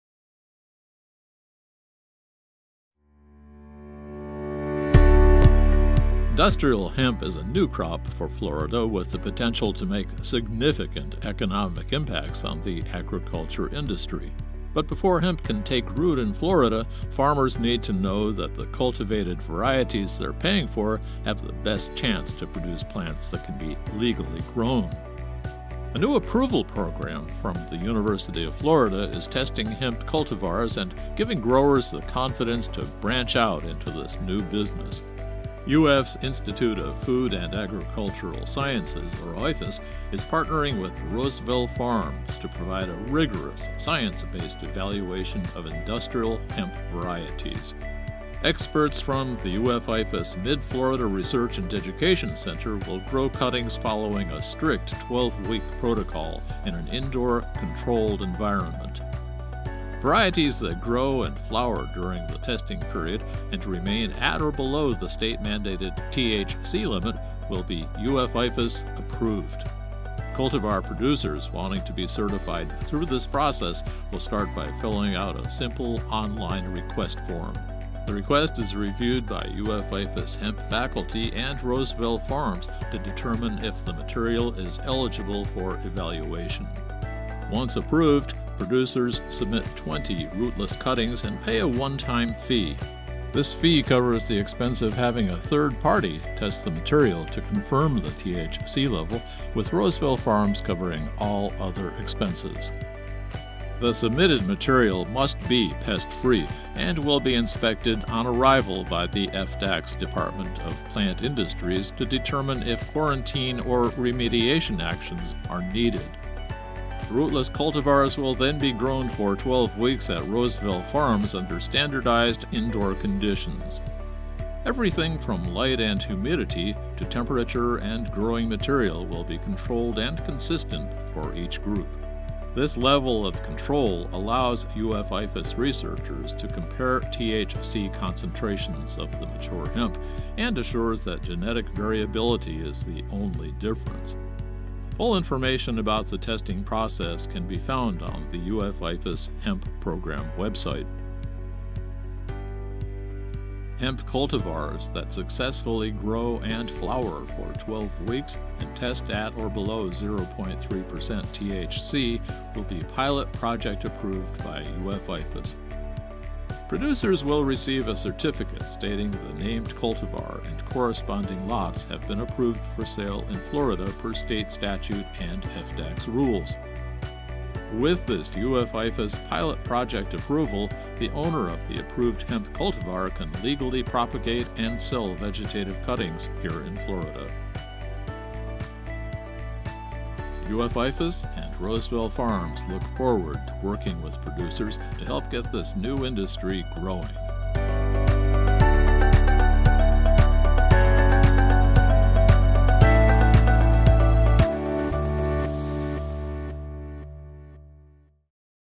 UF/IFAS Industrial Hemp Approval Program
Learn about the process UF/IFAS researchers are using to certify industrial hemp cultivars for sale in Florida.
For more information on the UF/IFAs Industrial Hemp Research Program visit: programs.ifas.ufl.edu/hemp